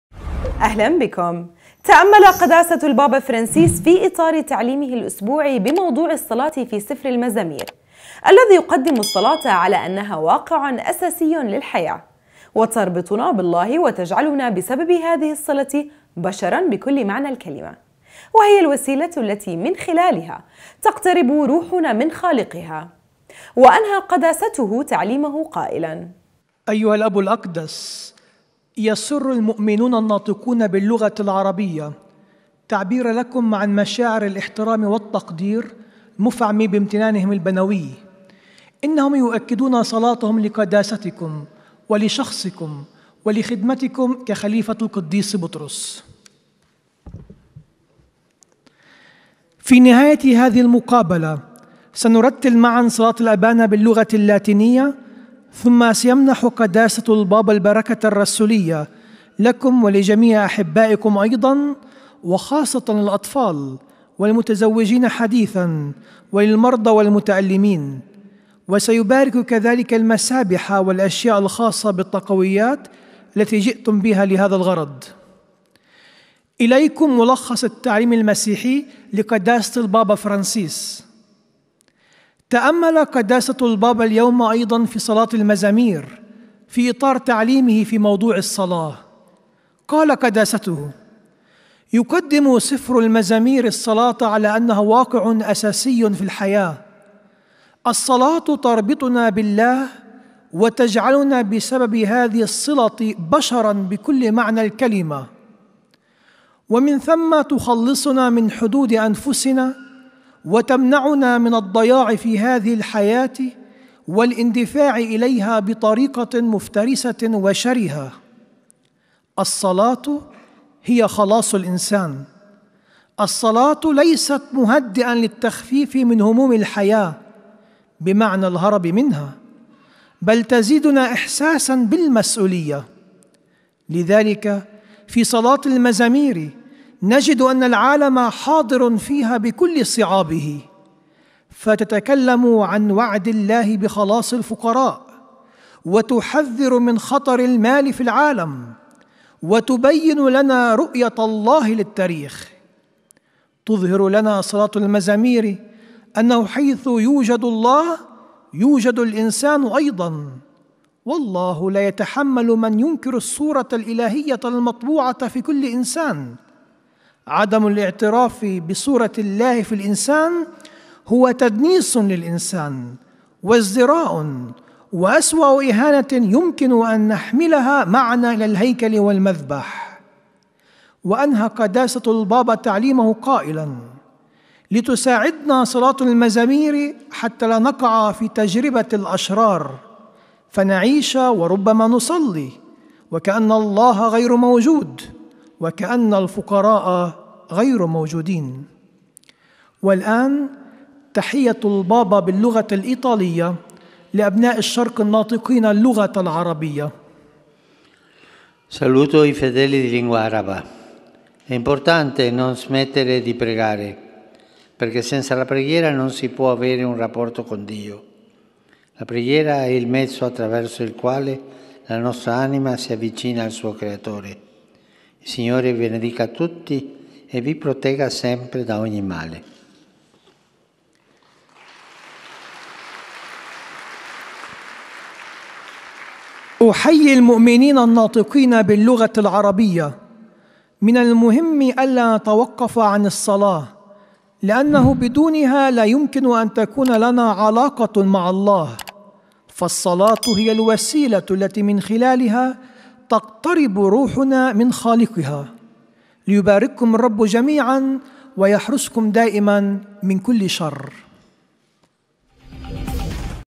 نورسات الأردن : البابا فرنسيس في لقائه الاسبوعي يؤكدُ أنَّ الصلاةَ هي التي تُقربُ روحَنا من خالقِها
تأمل قداسةُ البابا فرنسيس في إطار تعليمهِ الاسبوعي بموضوع الصلاة في سفرِ المزامير / الذي يُقدمُ الصلاةَ على أنها واقعٌ أساسيٌ للحياه / وتربطُنا باللهِ وتجعلنا بسببِ هذه الصلةِ بشراً بكلِ معنى الكلمه / وهي الوسيلةُ التي من خلالها تقتربُ روحُنا من خالقِها / وأنهى قداستُه تعليمَه قائلا :